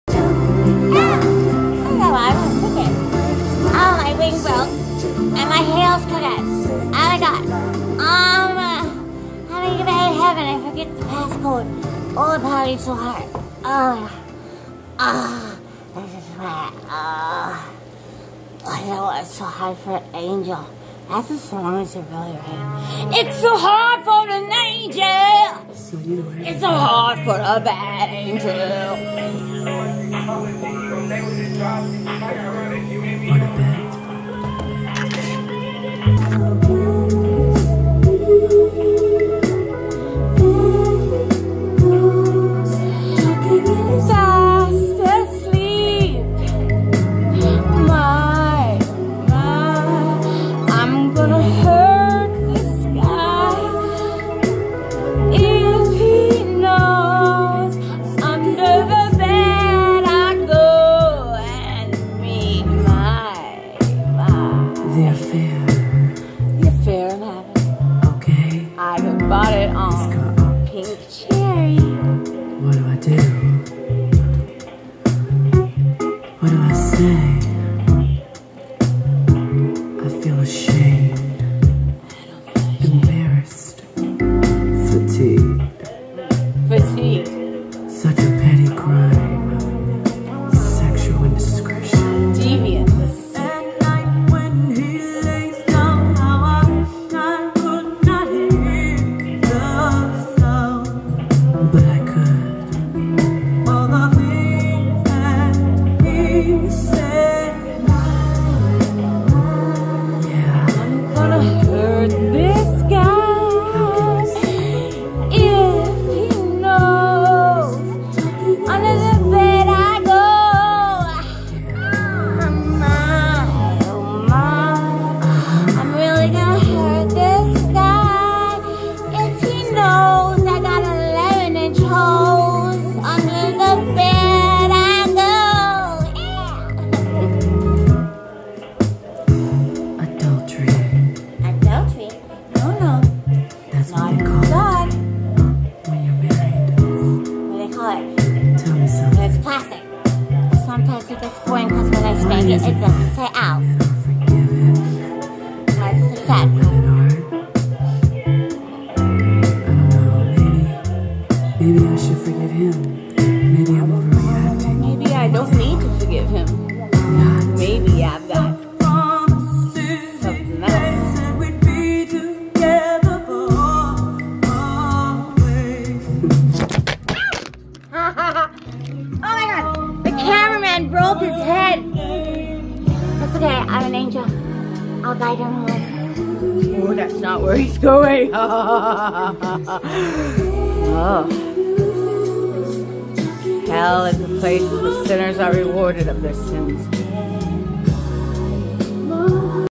AlienAngel Forgets the Passcode to Heaven! Learns all about DEVIANCE Tries her Hand At FLIRTING
Video from Kyre Traffod

*FORGETS THE PASSCODE TO HEAVEN

~DEVIENCE, SEXUAL INDISCRETION AND DOWNRIGHT FLIRTING. IS SO MUCH FUN. UNTIL.
AlienAngel has broken into humans home to learn more she's so curious about humans. She's trying her hardest to live up to the mission that God sent her to Earth for.. an act of attrition. In her quest to find answers and please her God, she knocks over the nanny cam. the radio turns on the TV turns on the lights in the house turn on... she's on camera now.,(and she can't get enough).She's still not perfect she's so Vain.
The lady singing from the box, talking about 'SEXUAL INDISCRETION'??! OH MY!!!
AlienAngel has a few secrets of her own in that department. She's still got her partly human self before. She's having so much fun. She's partying so hard she forgets the passcode to heaven. She's still a klutz her Halos bent, her left wing is broken at the tip (so she's flying in circles)
she knocks over the nanny cam, a teddy bear, and she's finally of service! She jumps at the chance to show God how good she is and bring him to heaven. Plus she figures she won't need the passcode.. if she follows him!!!
UNFORTUNATELY THAT'S NOT WHERE HE'S GOING.